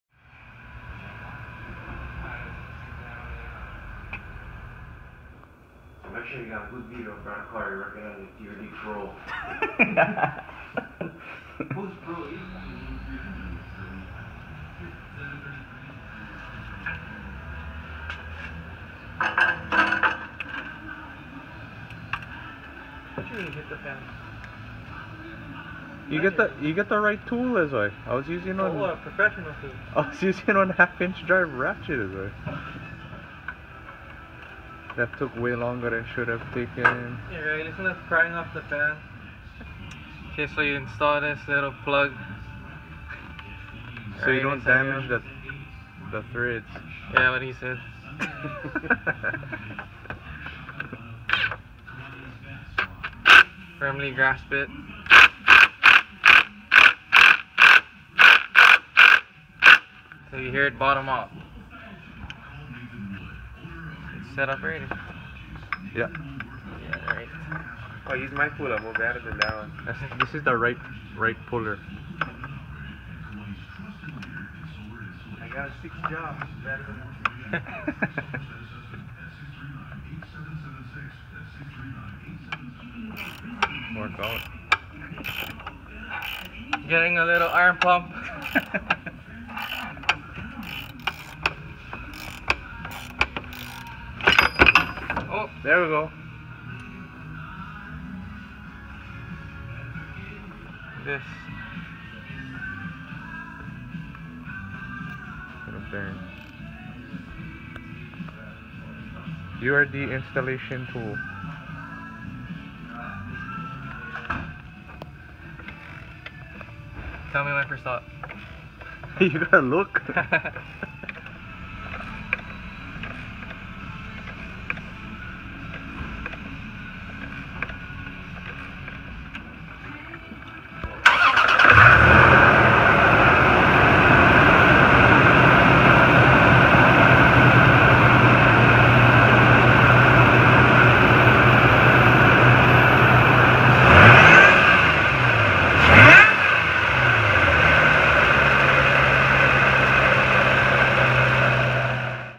URD 2.8 TRD Supercharger Pulley Installation At Kauai Auto Repair
Installation of a 2.825" URD Non-Stealth SuperGrip pulley on a 2013 TRD supercharged Tacoma. 4.0L, 1GR V6.  This pulley will produce approximately 1.5-2 PSI of additional boost. Technician:  Aaron Imada. Owner:  Stan Kua.  Shot with an iPhone 7.